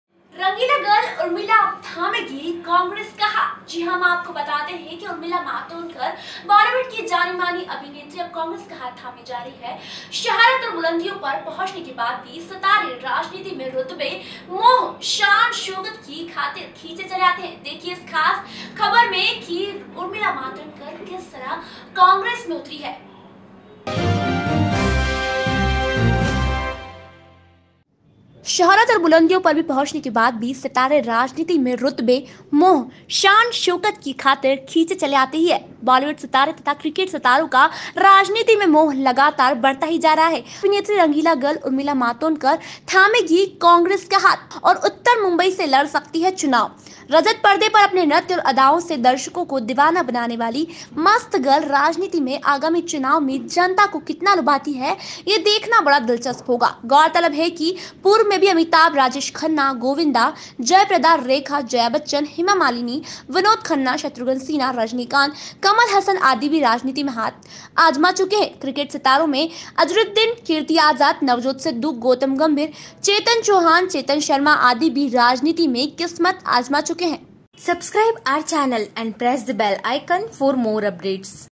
LokSabha Elections 2019: CONGRESS में क्यों शामिल हुईं अभिनेत्री Urmila Matondkar
LokSabha Elections 2019: CONGRESS में क्यों शामिल हुईं अभिनेत्री Urmila Matondkar? Glamour Girl Urmila Join Congress...